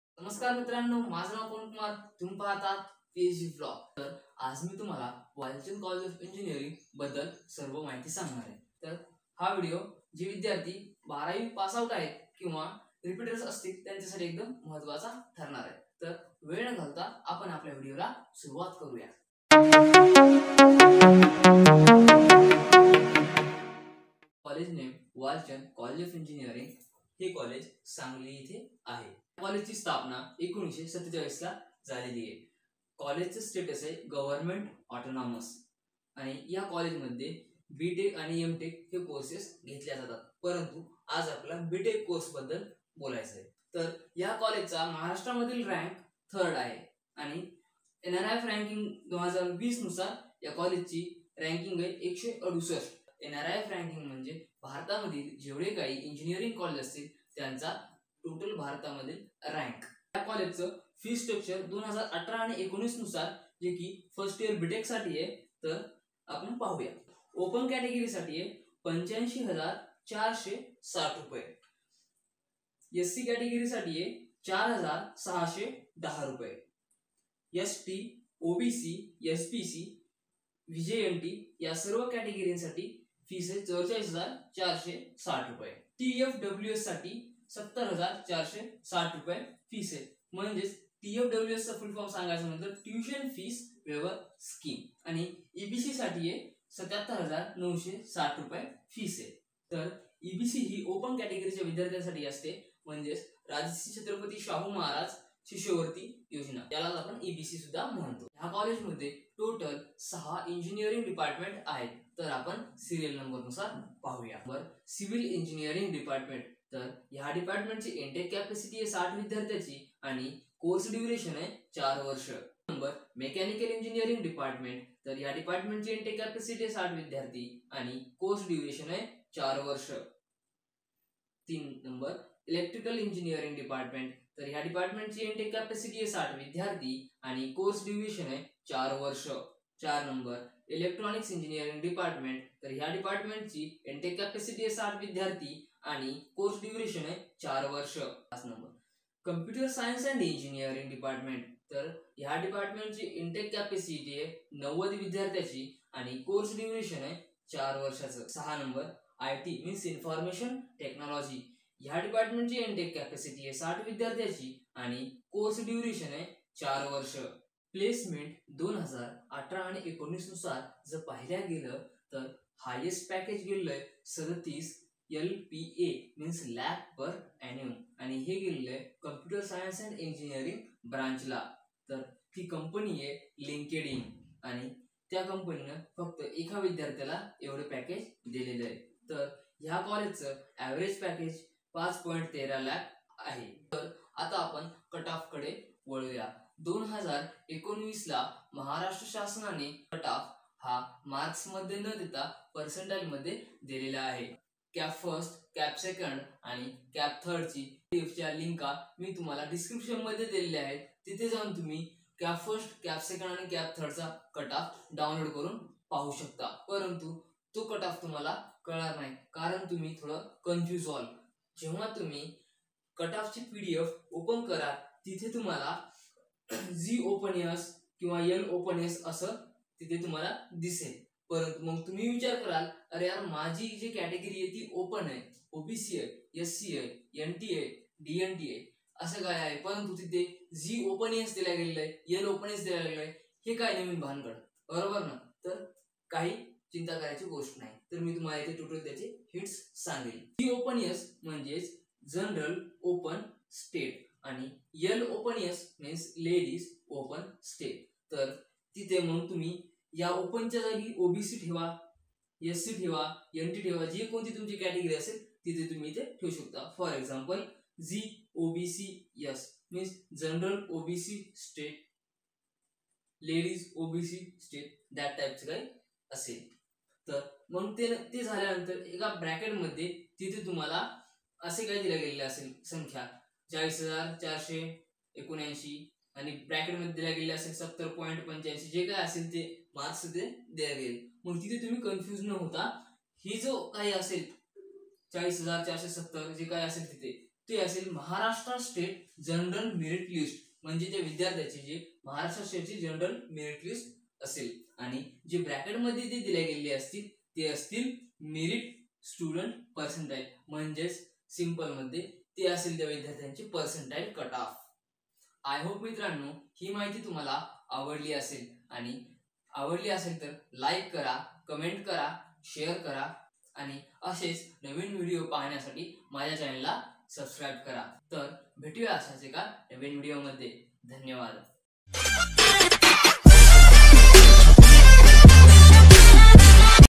Walchand College of Engineering, Sangli | Fees Structure | Placement | Cutoff 2019 In Marathi
Note : PSG VLOGS Channel Name Has Been Changed To Pavankumar Gajwe.
सर्व भारत वासीयांना Pavankumar Gajwe तर्फे 74 व्या स्वातंत्र्य दिनानिमित्त हार्दिक शुभेच्छा...! PavankumarGajwe.
Above All College Information Is Taken From College official website and collegepravesh website.
This College Established In 1947. Walchand College of Engineering is situated midway between Sangli and Miraj cities at Vishrambag, Sangli. The WCE campus is located on about 90 acres of land on southern side of Sangli – Miraj road.
College Location : Sangli
In This College 6 Engineering Departments.
1) Civil Engineering
2) Mechanical Engineering
3) Electrical Engineering
4) Electronics Engineering
5) Computer Science and Engineering
6) Information Technology